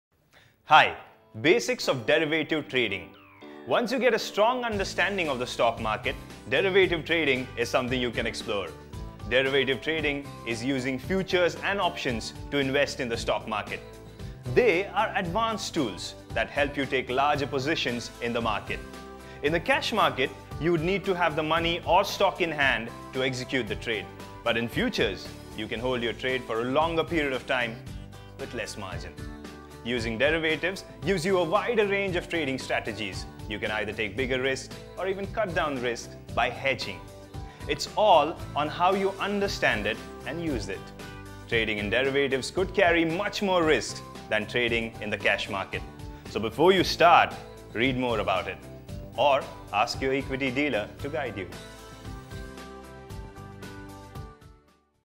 What is derivative trading and derivative investments by Kotak Securities.
Derivative investment is an option with many tools to explore a larger portion of the market. It is advisable to invest in derivatives after an understanding of the stock market completely.
It gives you an option to invest in future, take a bigger risk and cut down by hedging.

Watch the video to know more about what is derivative trading.

To understand what is derivative trading?, read the Knowledge Bank by Kotak Securities by clicking the